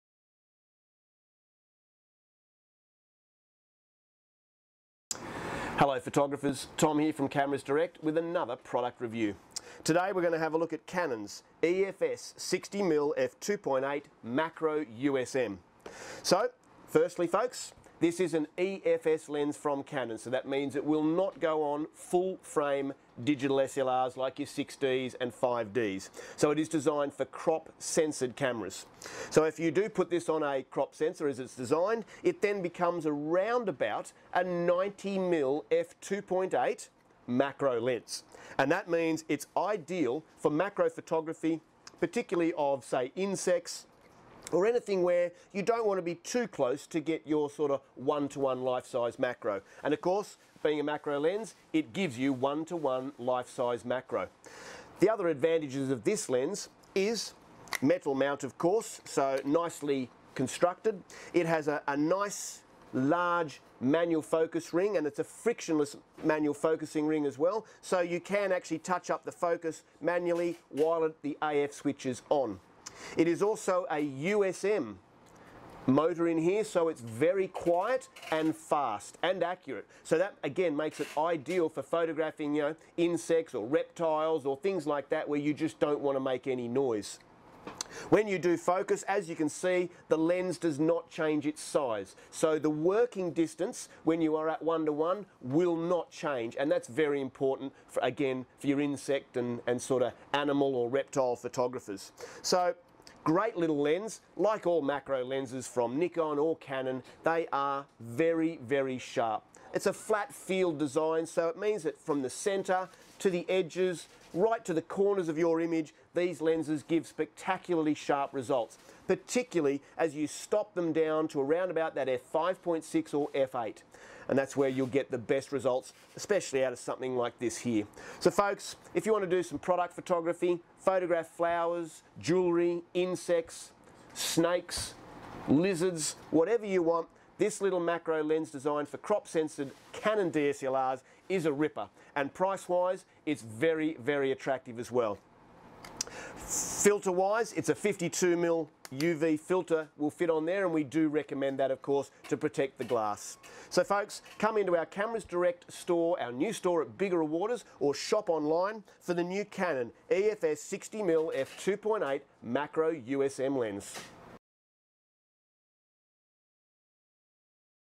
Canon 60mm EF-S f/2.8 Macro USM Lens Review | Cameras Direct Australia
Cheaper than the 100mm options and yet it could still be the best Canon macro lens option for you .. find out ..